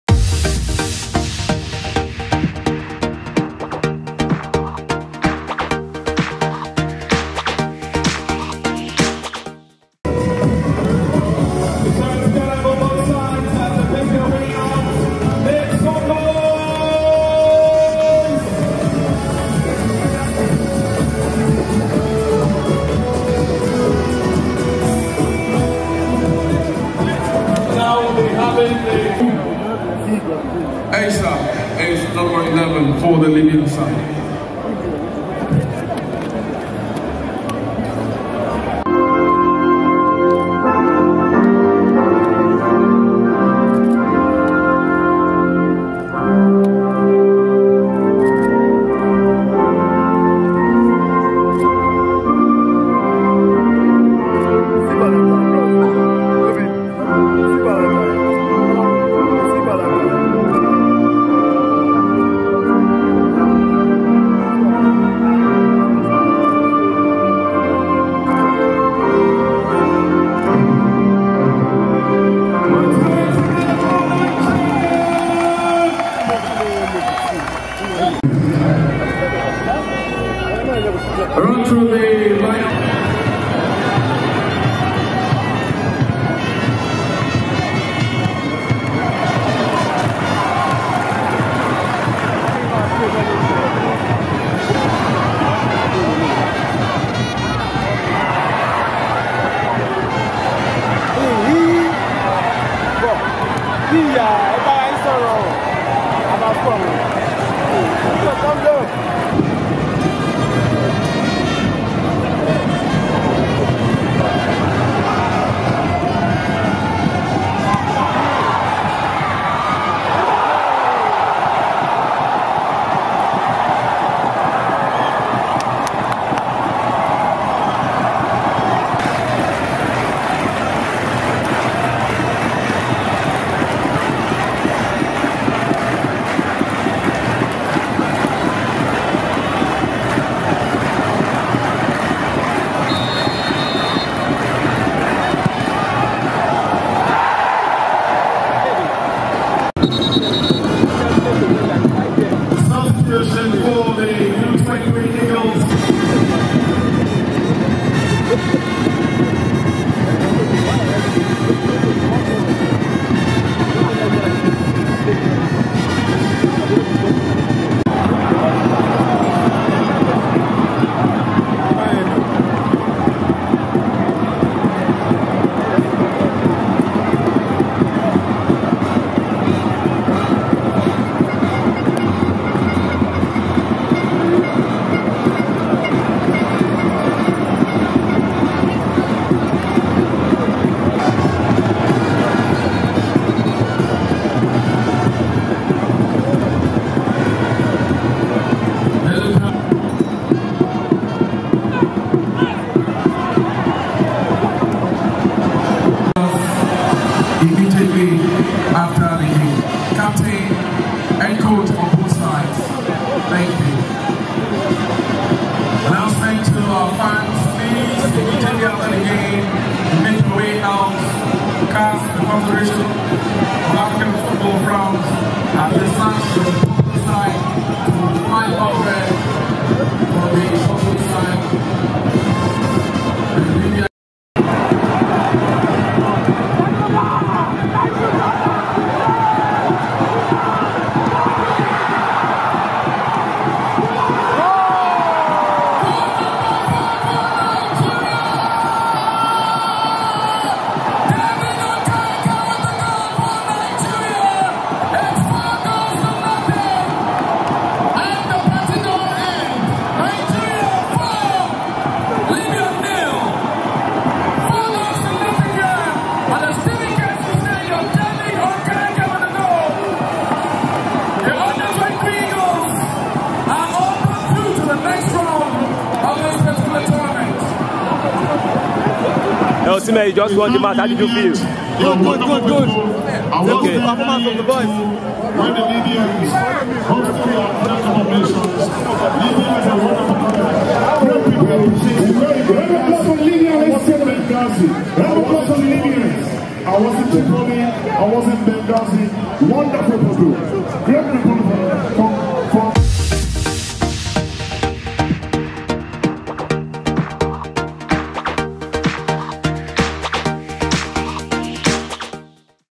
U-23 AFCON: Osimhen Scores Hat Trick As Nigeria Defeats Libya 4 0 To Qualify
Nigeria’s Under-23 team crushed Libya 4-0 in the second leg to progress to the final round of the Under-23 Africa Cup of Nations qualifiers.

Victor Osimhen's expertise and efforts became highlighted as he grabbed a hat-trick, which contributes to Nigeria's qualification.

Watch this video with peace of mind.